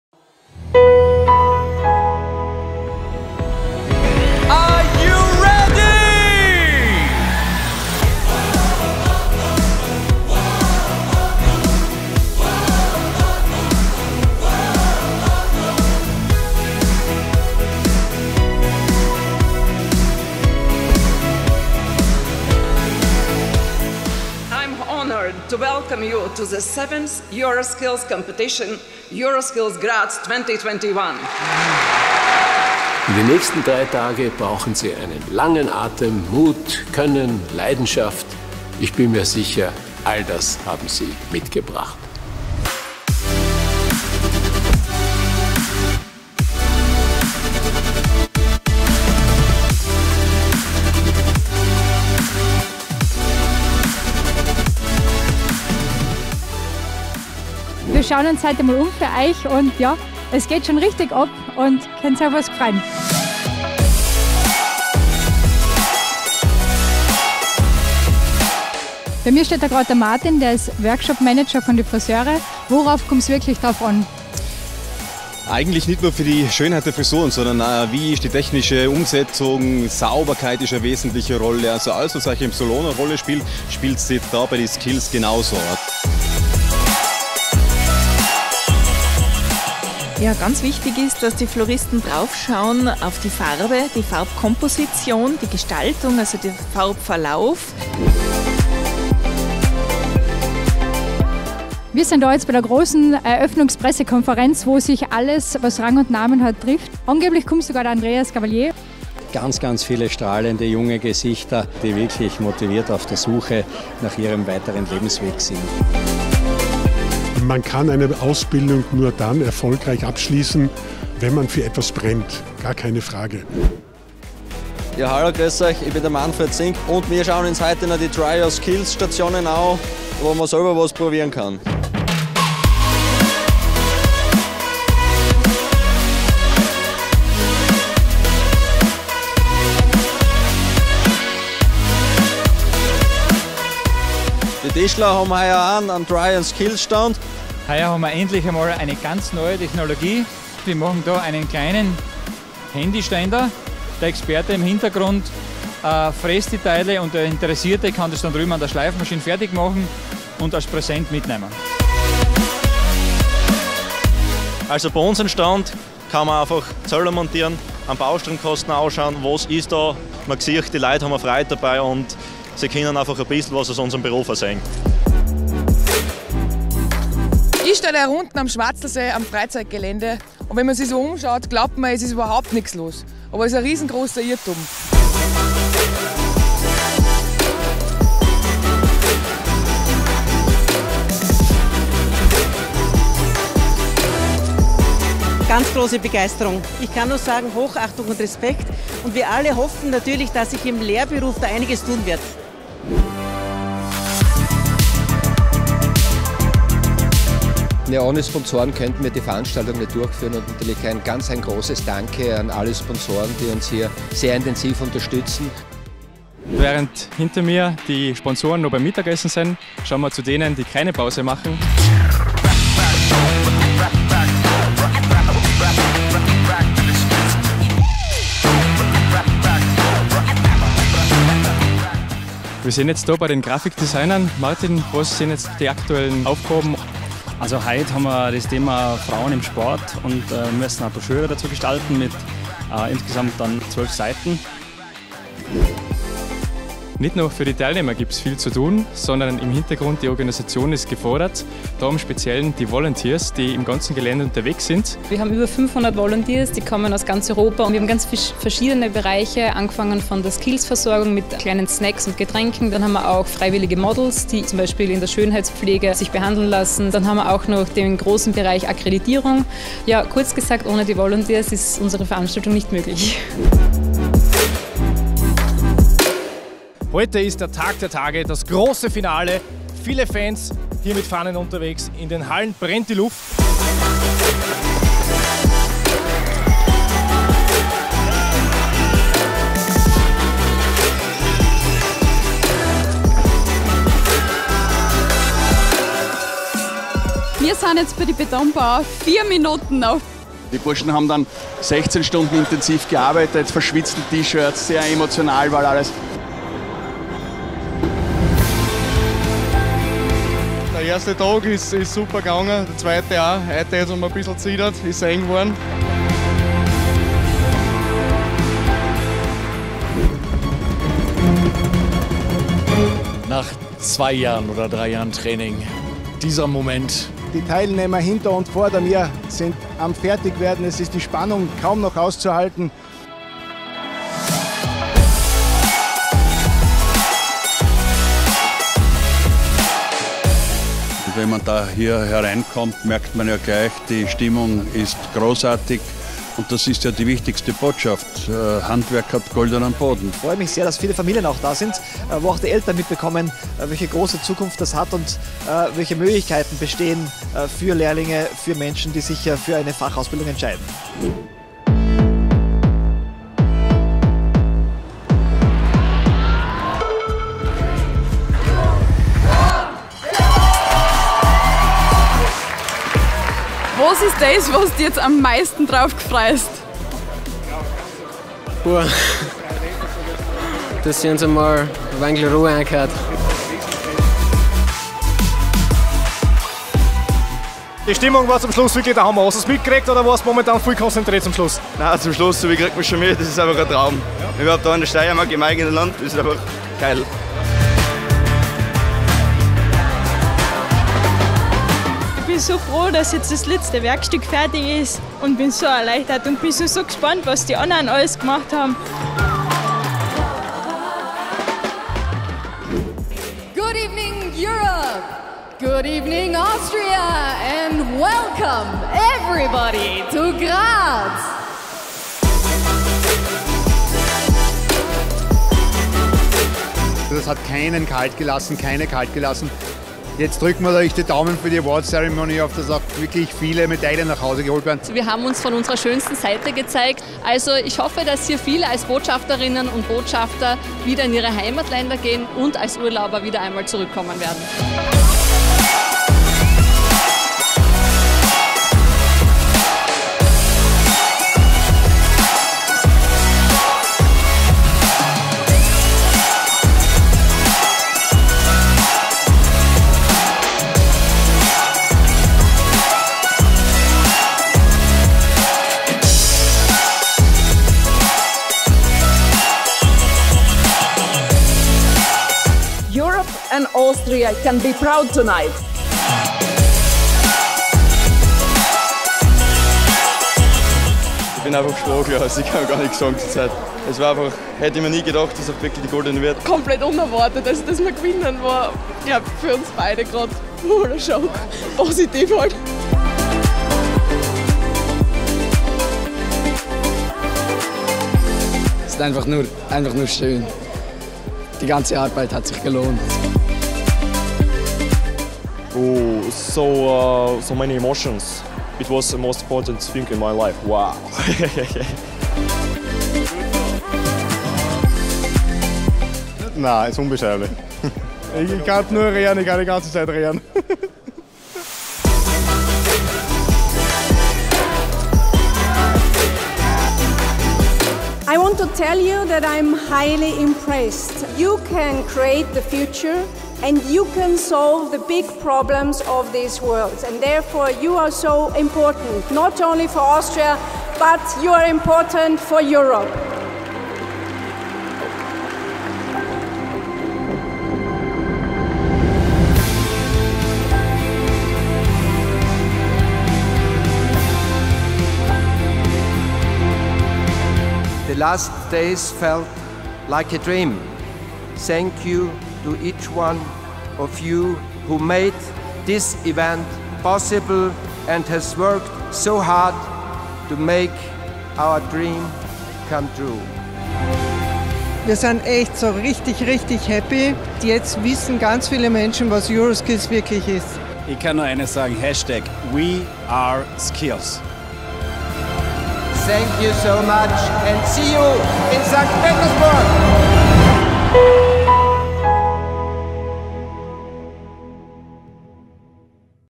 EuroSkills Graz 2021 - Event Highlights
EuroSkills - the biggest vocational education and skills excellence event in Europe - was held in Austria for the very first time in 2021.

At EuroSkills Graz 2021, more than 400 young skilled professionals from across Europe came together to participate in 38 different skill competitions – all for a chance to be recognised as the Best in Europe in their individual skills.

EuroSkills Graz 2021 welcomed more than 25,000 visitors, including 10,000 students, who had the opportunity to try hands-on job-related skills across a huge range of “Try-a-Skill” activity stands.